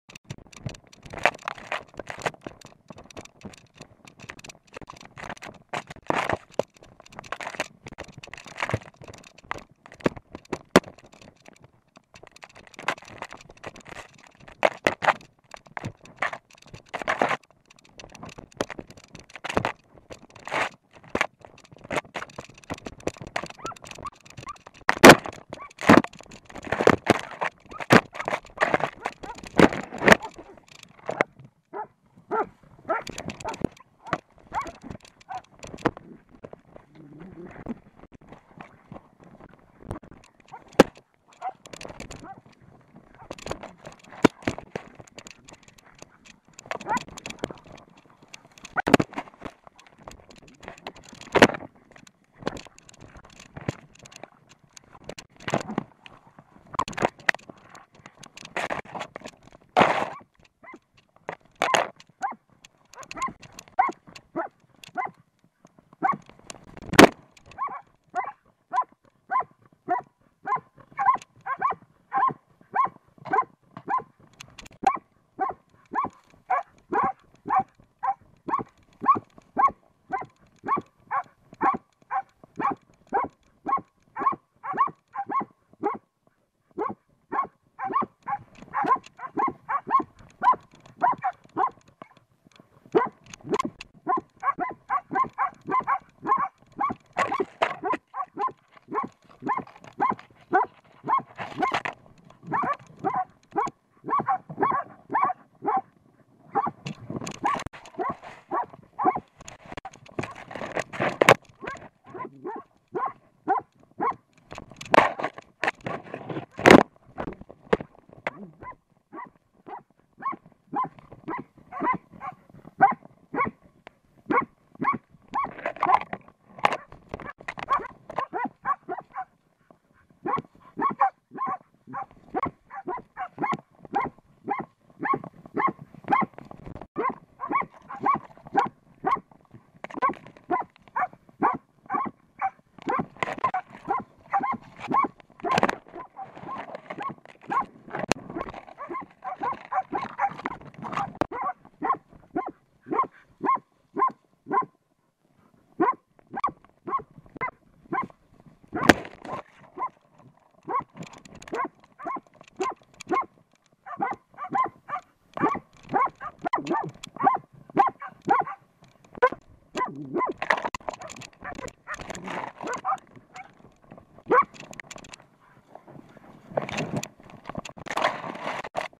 elk hound in face of badger, contour roam. elghund i slåskamp med greving. dog fighting
elk hound inn fight with a dadger.
elghund i slåskamp med grevlingl